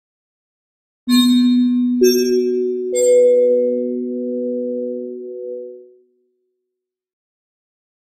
NET 2019 Custom ID
Last year, a re-imagined NET ID surfaced. but without the antenna on the top of the roof. This fixes that.